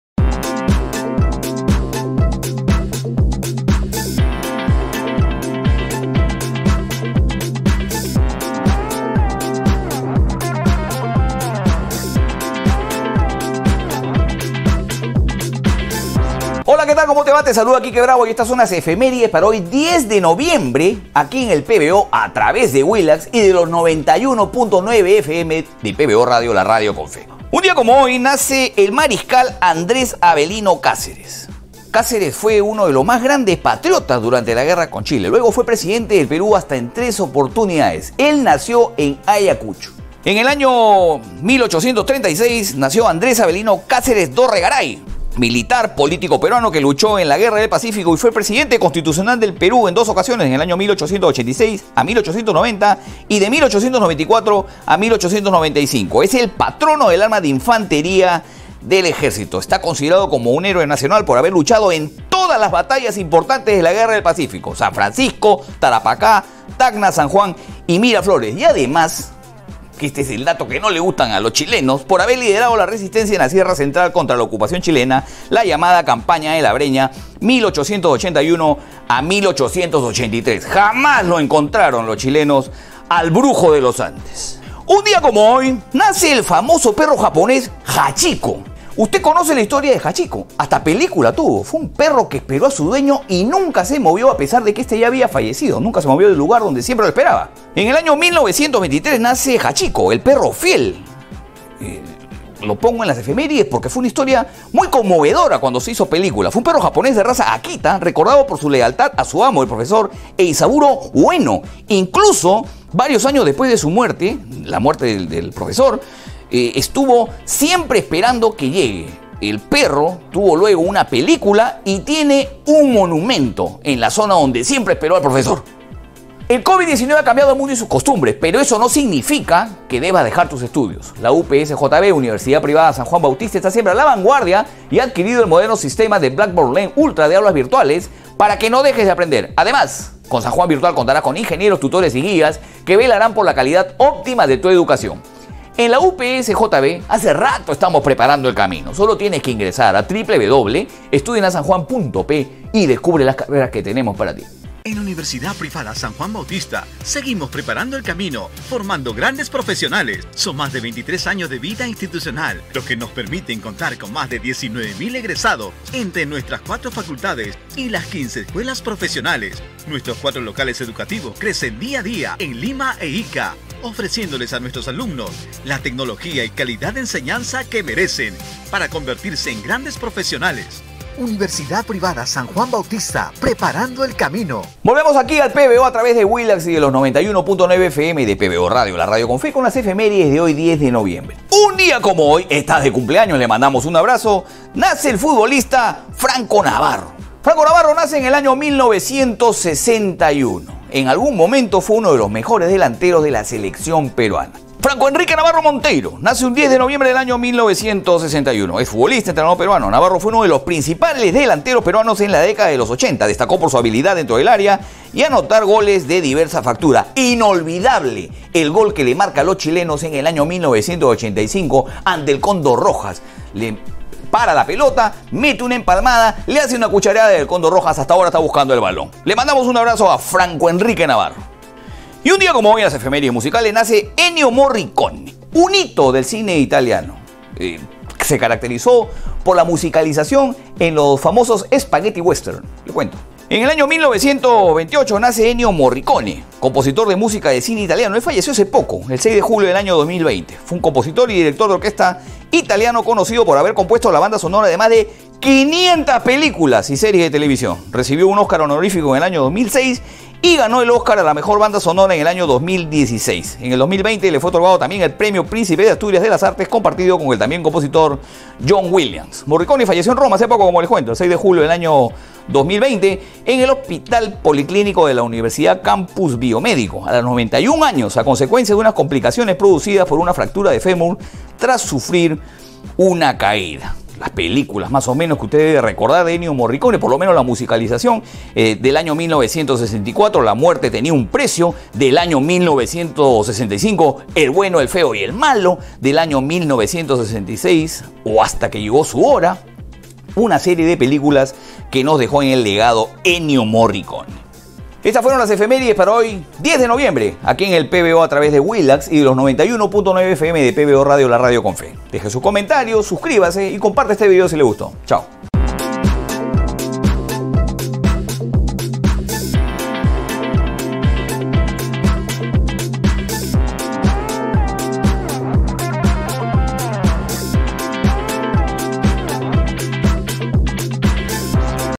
Efemérides 10 de noviembre
Nace en 1836 el Mariscal Andrés Avelino Cáceres... Está de cumpleaños el ex futbolista Franco Navarro...